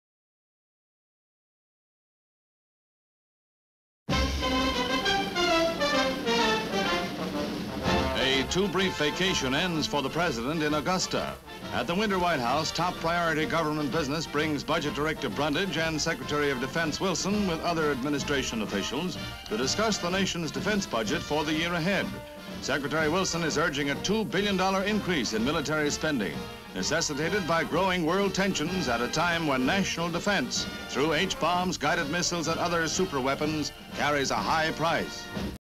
Wilson urges $2 billion increase in defense budget during a defense budget talk a...HD Stock Footage
Wilson urges $2 billion increase in defense budget during a defense budget talk at the Winter White House in Georgia.

US President Dwight D. Eisenhower during a defense budget talk at the Winter White House in Augusta, Georgia. A sign reads 'Augusta National Golf Club'. The Winter White House. US Budget director Percival Brundage, US President Eisenhower, US Secretary of Defense Charles Erwin Wilson and administrative leaders seated around a table. They discuss nations future defense budget. Wilson urges $2 billion increase in the budget. The members discussing the budget. A close up of Eisenhower. Location: Augusta Georgia. Date: December 10, 1956.

57,000+ broadcast-quality historic clips for immediate download.